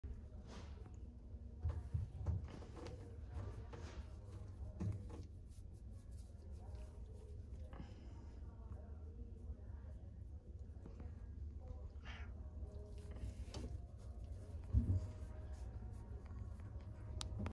Video of adoptable pet named Merlin
Merlin is an adoptable pet with Give Me Shelter Project in Flushing NY.